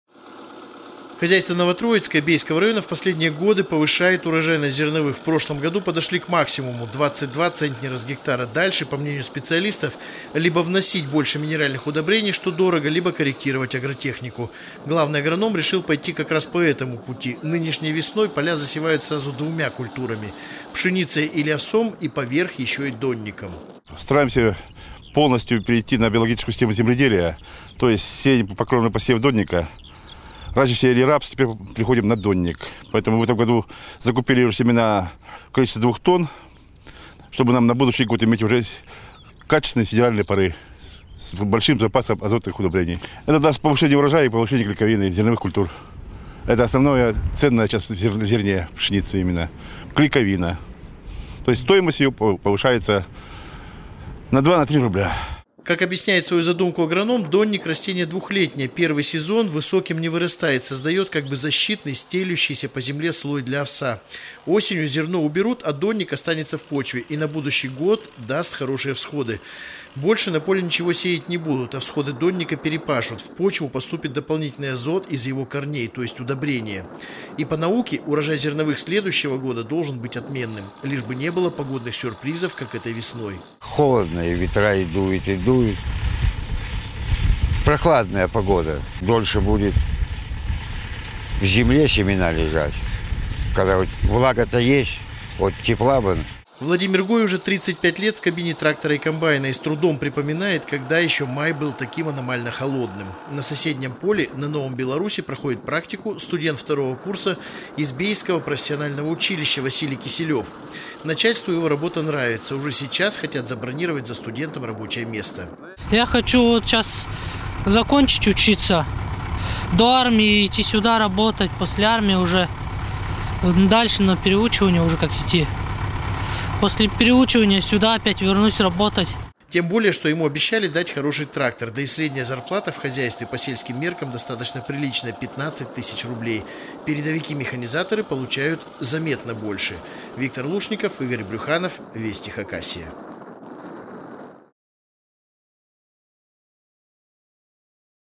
Биотехнологии в зернопроизводстве 13.05.2016
Их начали внедрять в одном из хозяйств Бейского района. Это позволит повысить качество зерна и его товарную стоимость.